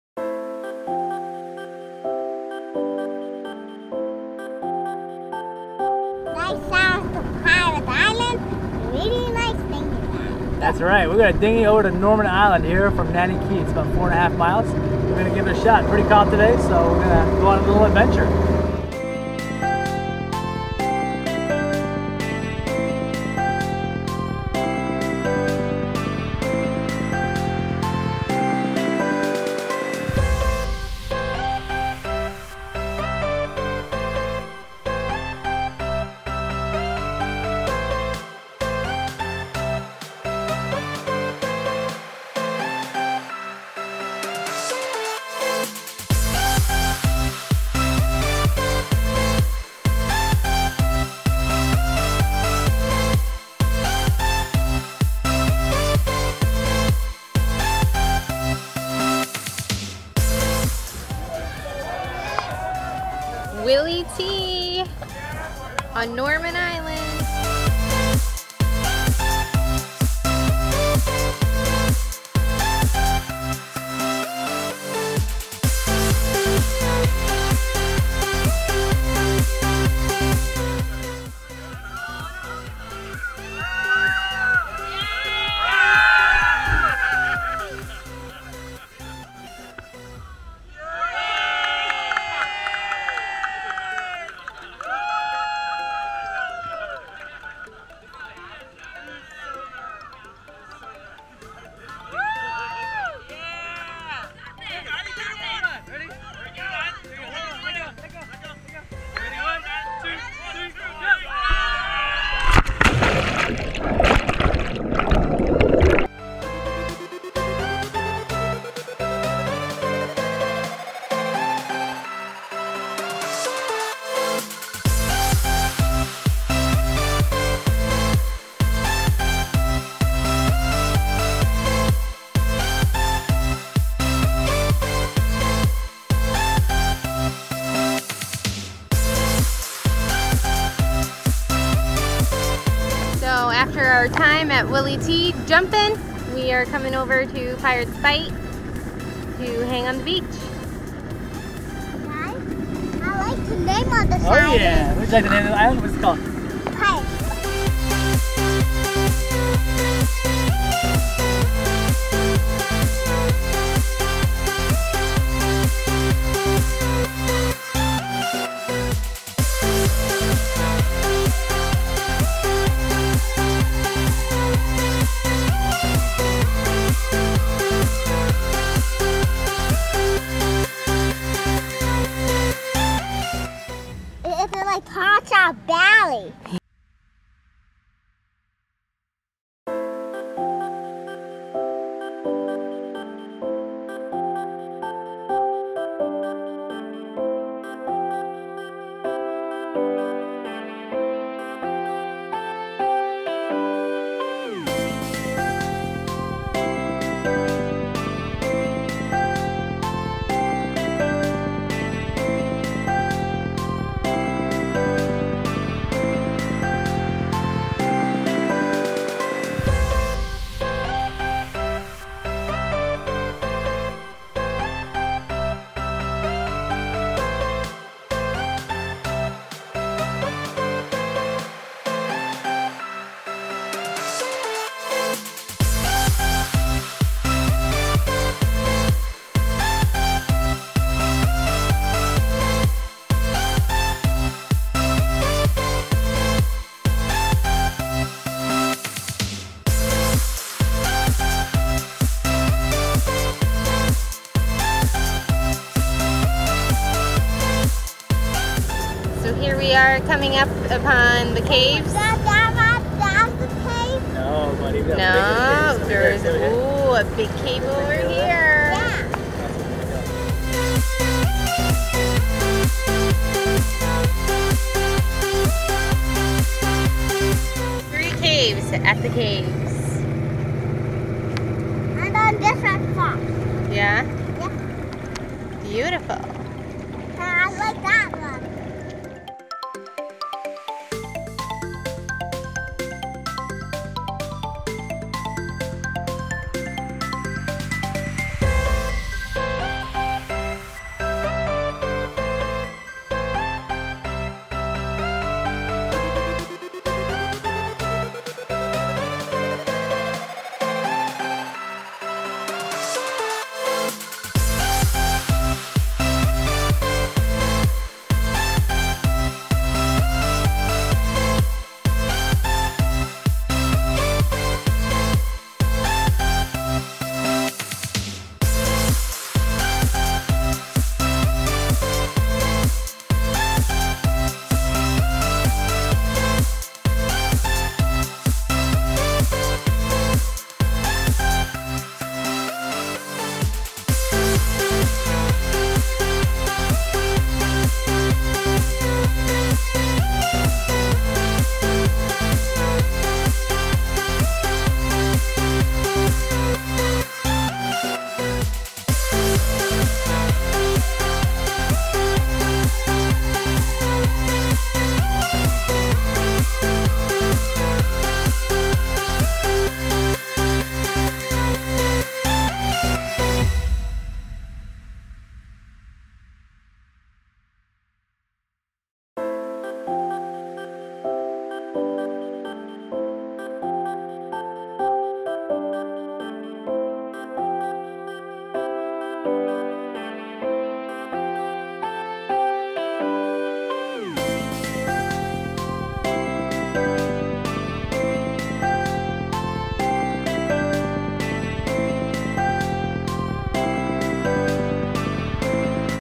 15. Norman Island, BVI(Sailing Sunkissed)
Luuck has been busy working- making us that moola for our cruising kitty! It has left us little time to explore so on his day off we put the dingy down and hightailed it to Norman Island from Nanny Cay! We had lunch at Willy T and Stormer jumped from the top of the bar not 1...not 2. but 5 times that day(3 on camera)!!! Spent time on the beautiful beach and swam in caves!! It was a good day!! Enjoy!!